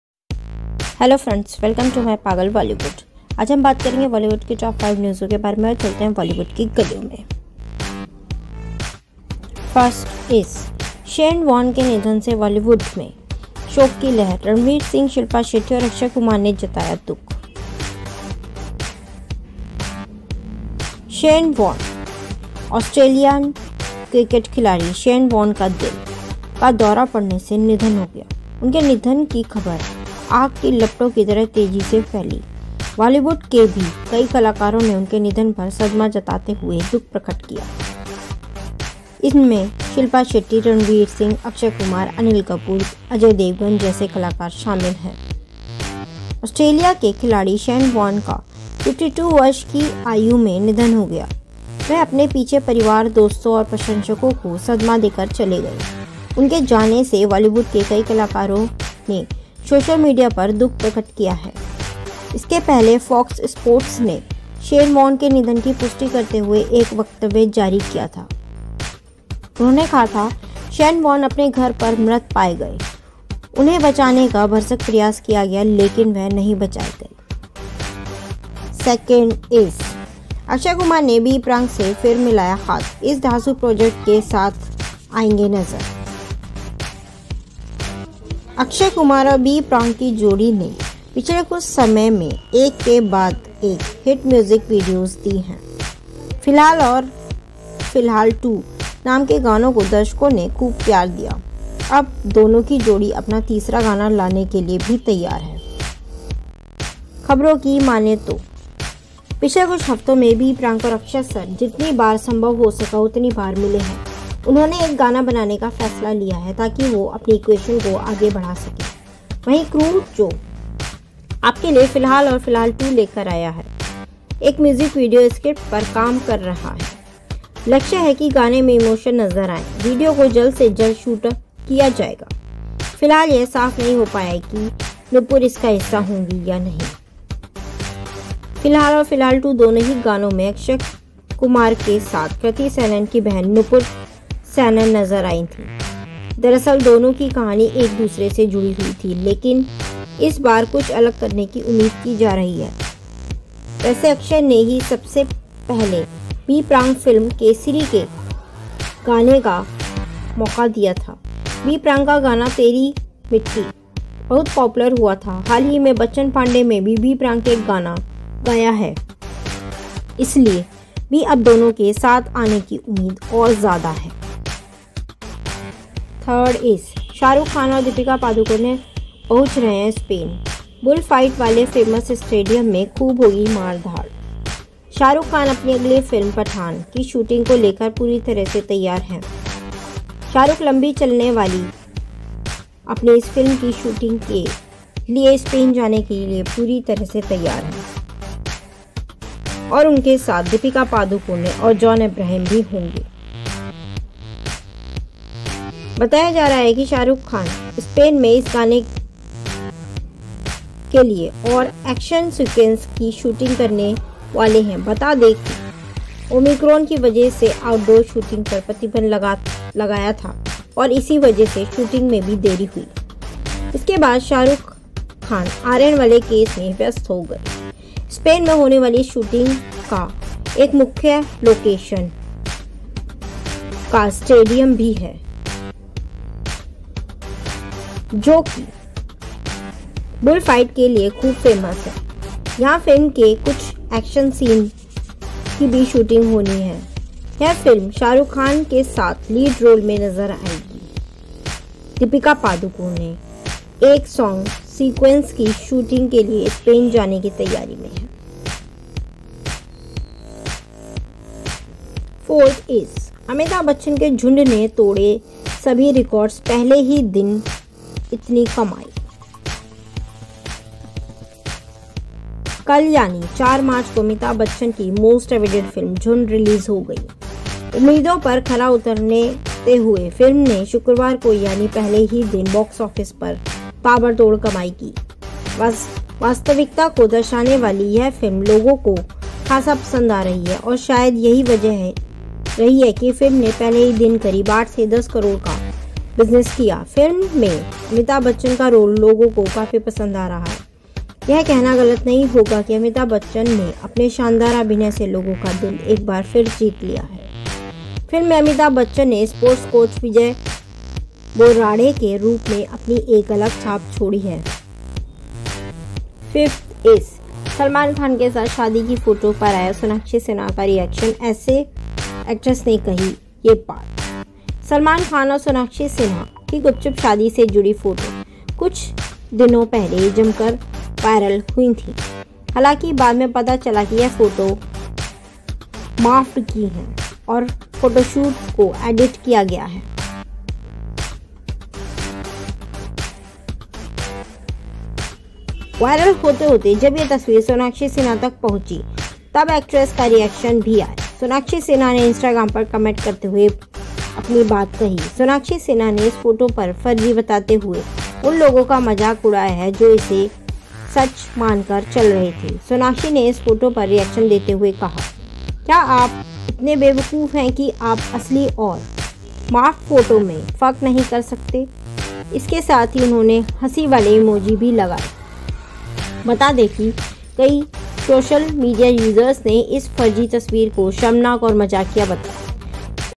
Top 5 Bollywood News||Jhundh ने तोड़े record||Sanjay Bhansali ne कहा salman मानेंगे तो करेगे साथ काम
Hello Friends
Welcome To PaagalBollywood